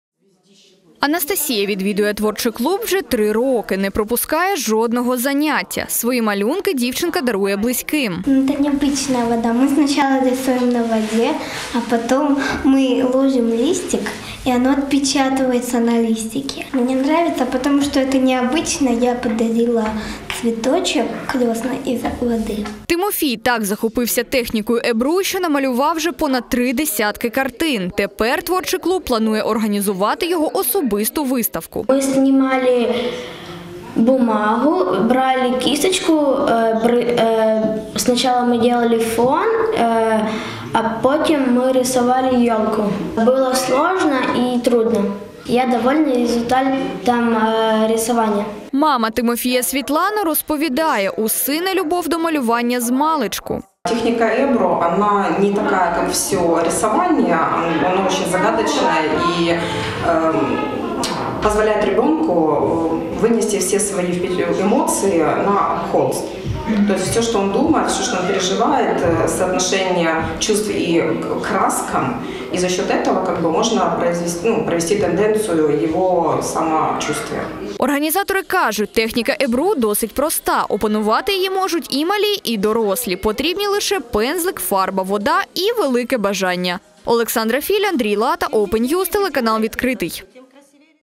У Дніпрі діти малювали картини технікою ебру
Магія на воді. У Дніпрі вихованці дитячого творчого клубу створювали справжні новорічні дива за технікою ебру, тобто малеча малювала зимові пейзажі фарбами на воді.
Подробиці новорічного дива дізнавалась наша Олександра Філь.